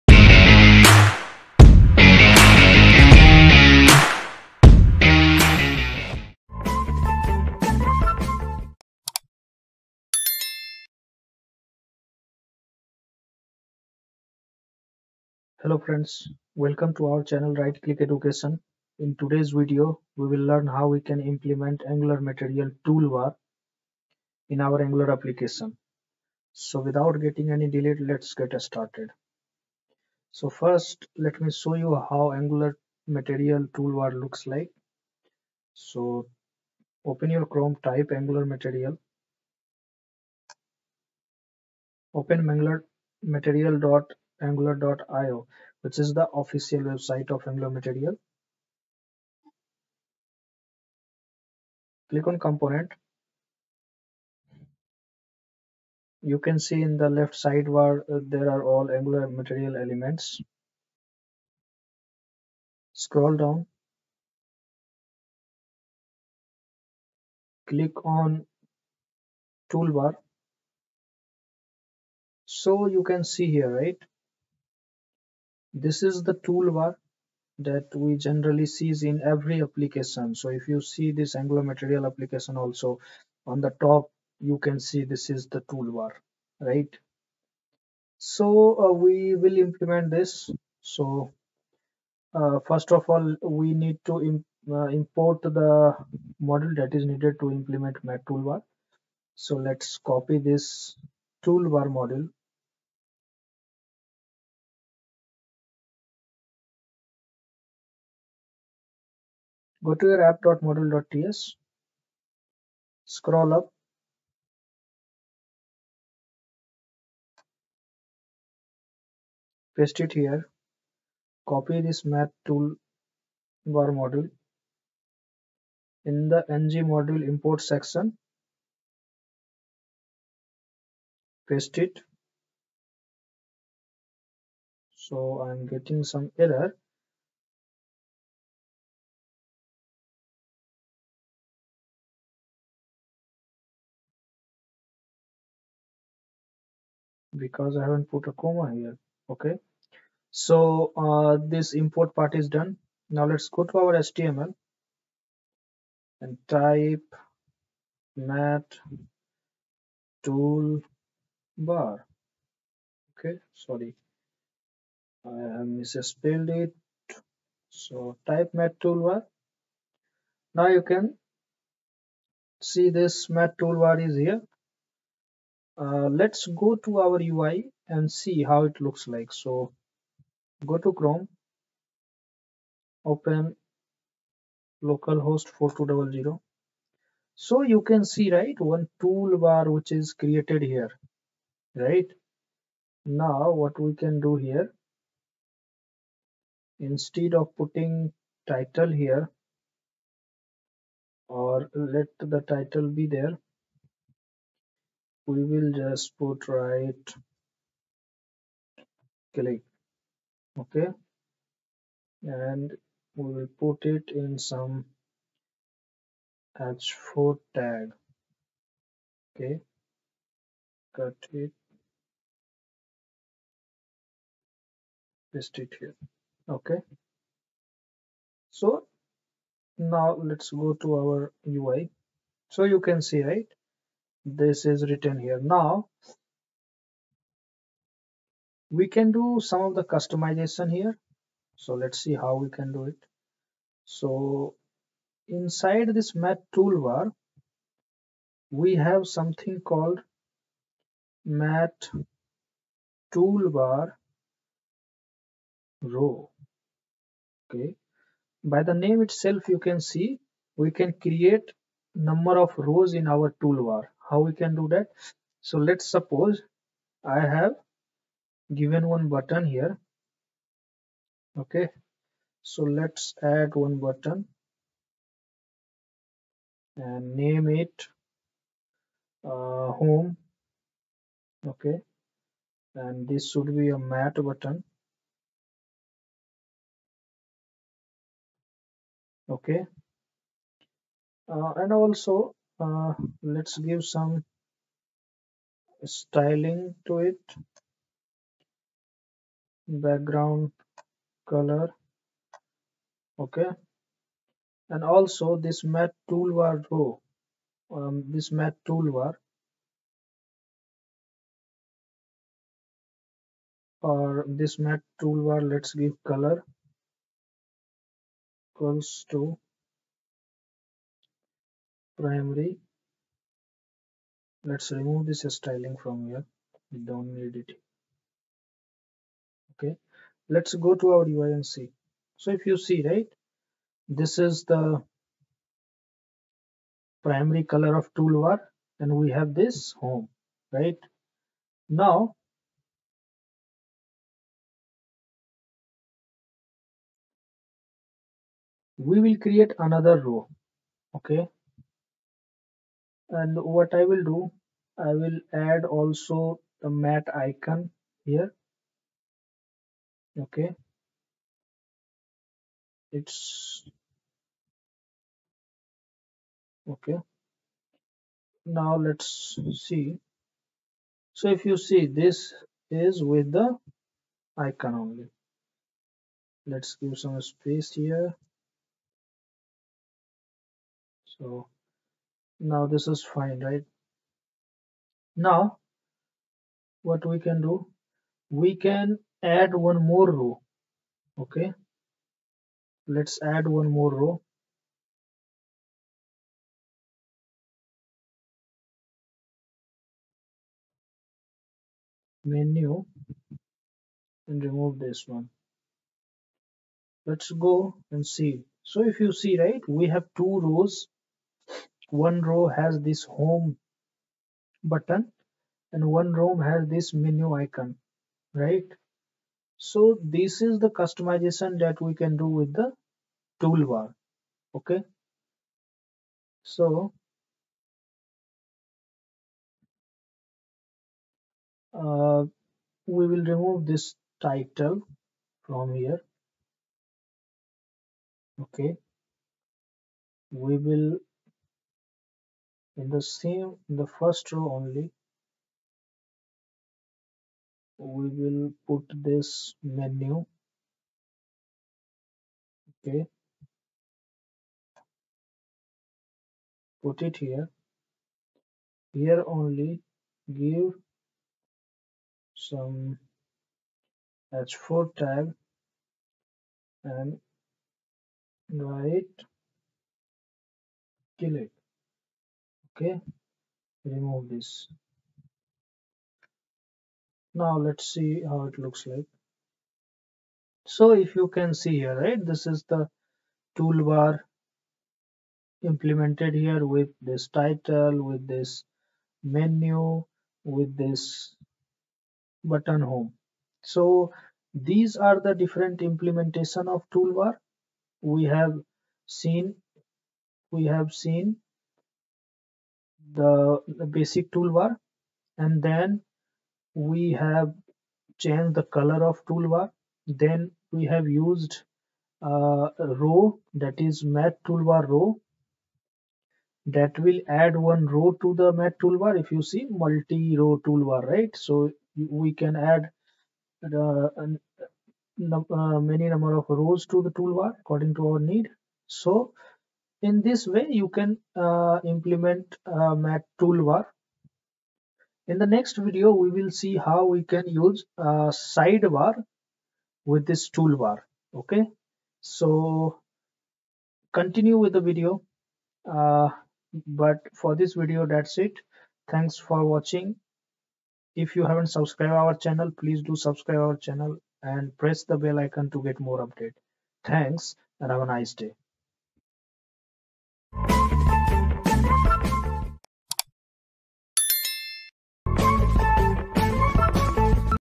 Angular material Toolbar | Angular material Tutorial 26
Step by step  tutorial on how to use Angular Material Toolbar. The Angular Material toolbar is an Angular directive, is used to show a toolbar which is normally an area above the content to show the title and the relevant buttons.

00:00 Intro video
00:16 Introduction to angular material toolbar
01:30 Basic mat toolbar Implementation.
03:57 Toolbar row implementation.
08:00 Summary

My Computer
My Smart Watch
My Mic